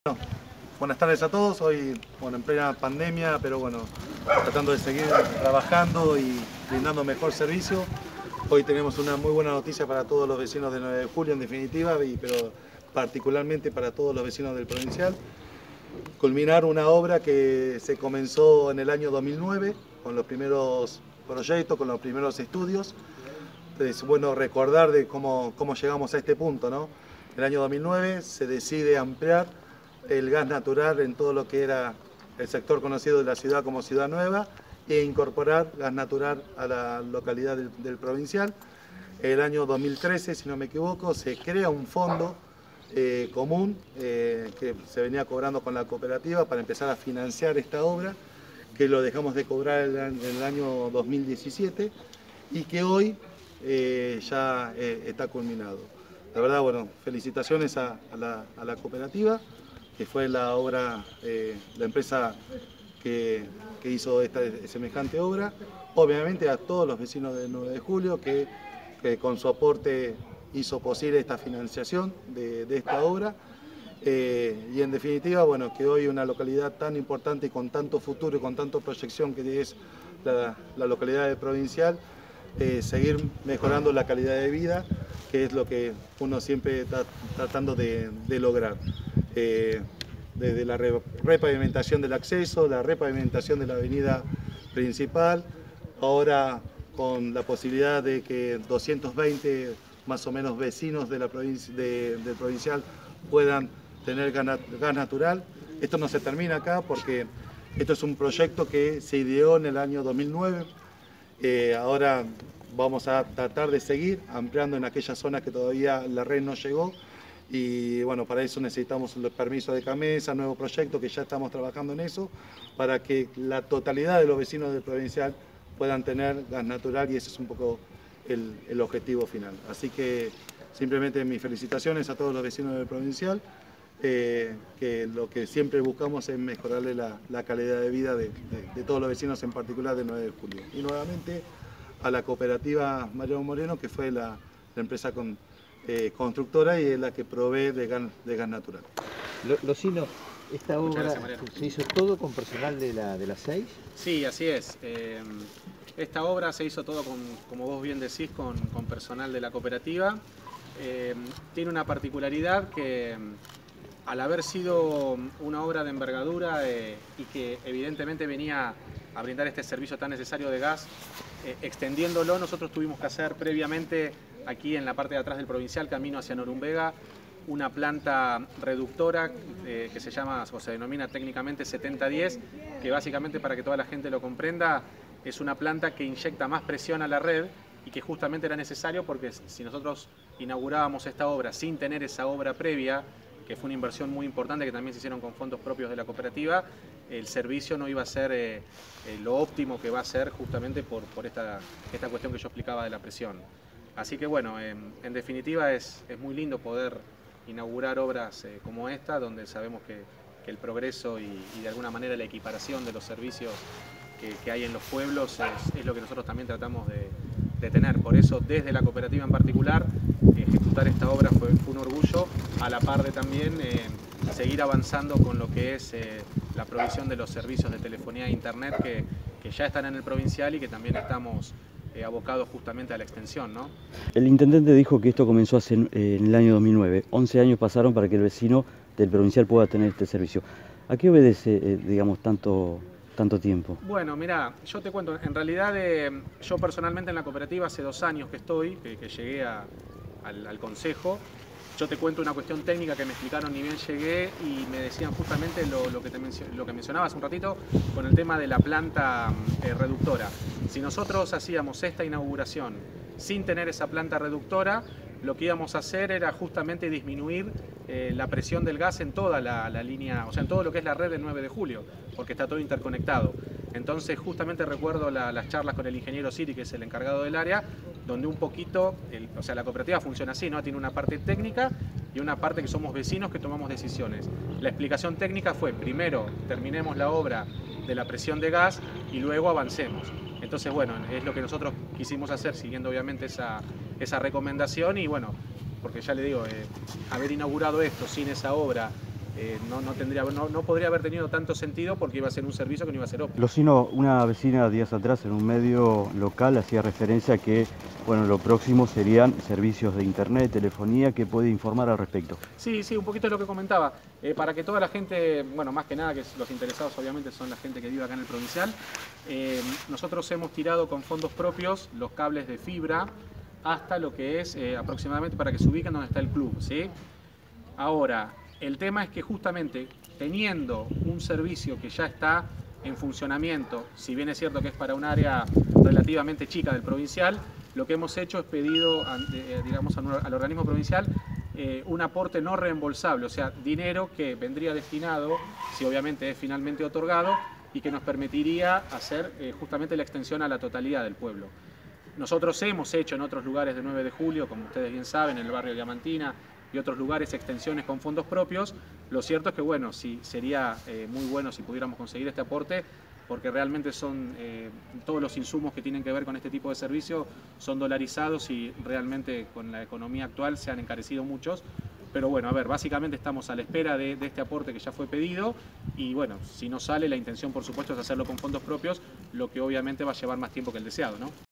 El Provincial ya cuenta con red de gas
El Intendente Mariano Barroso y el presidente de la CEyS Mariano Moreno, Matias Losinno dejaron inaugurado la obra que por el momento tiene una solicitud de  70 vecinos y tiene una capacidad de  mas de  200 familias y ya se esta gestionando para ampliar la misma.